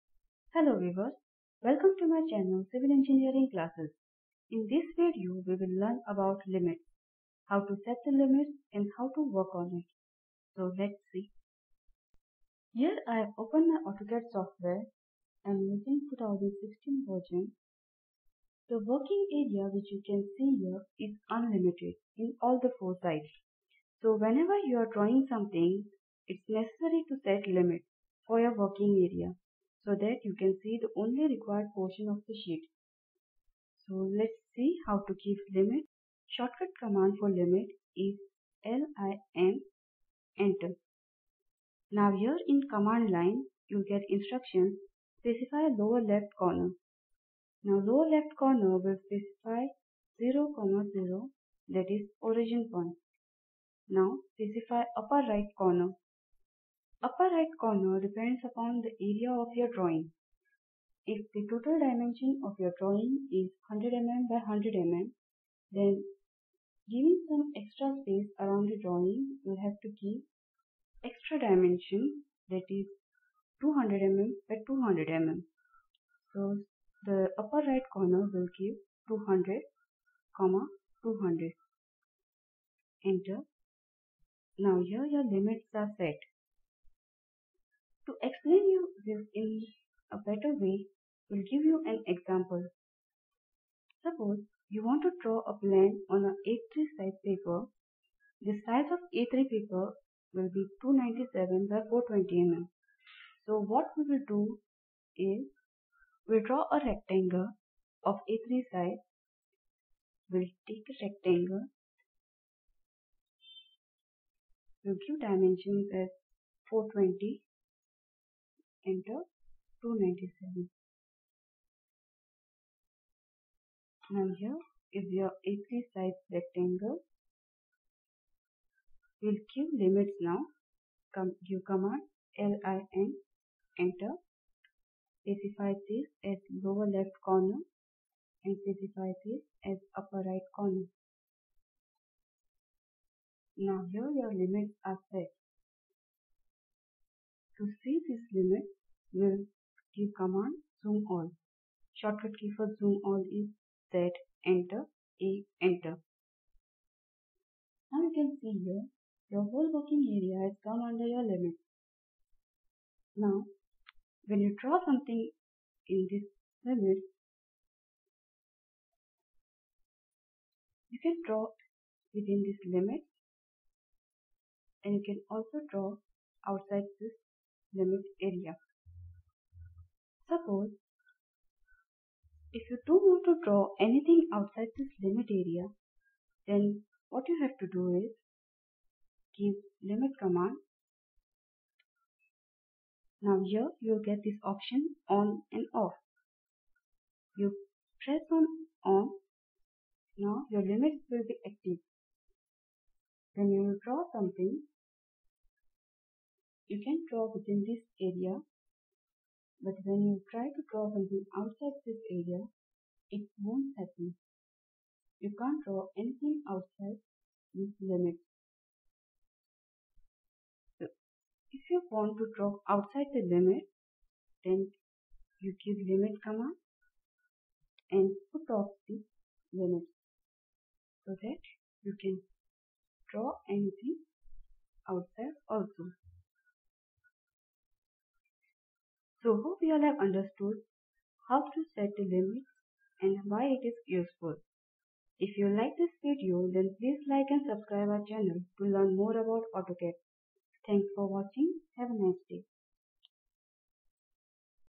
How to set limits in Autocad
Autocad beigneer

this video will give you details about how to set limits in Autocad
How to set limits
why its required to set limits
U will get all the information in this video